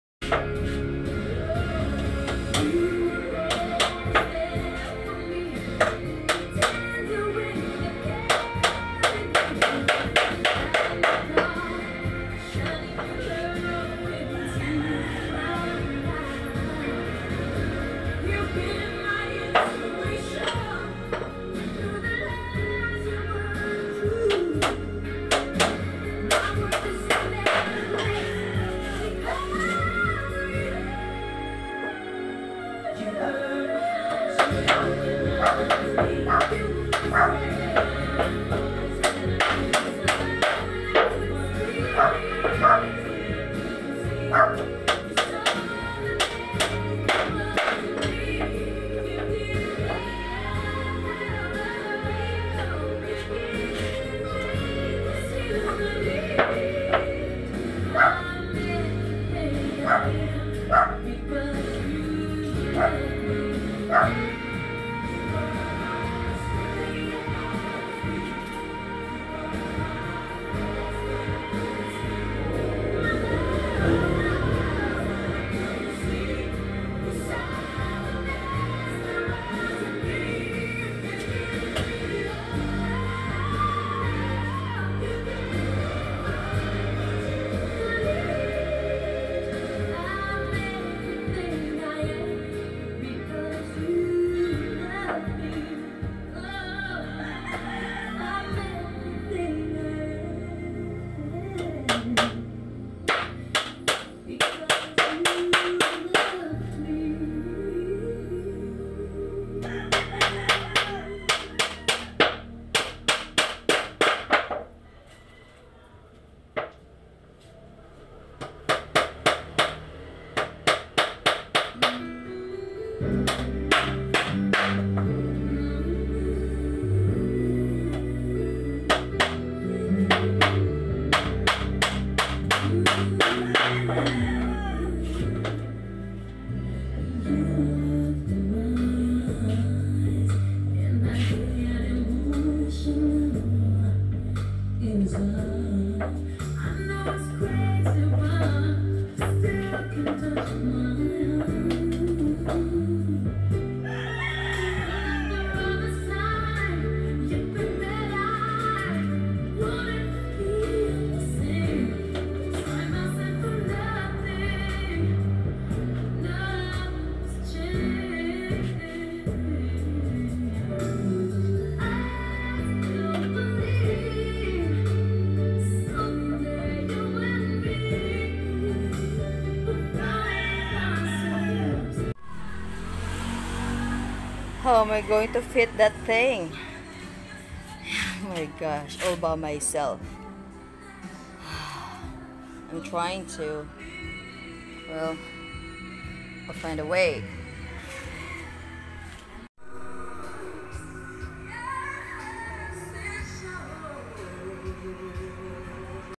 4x5Floor SweetFurBabies Trio LaborOfLove
the making of 4*5feet doghouse floor made of good lumber 😊💪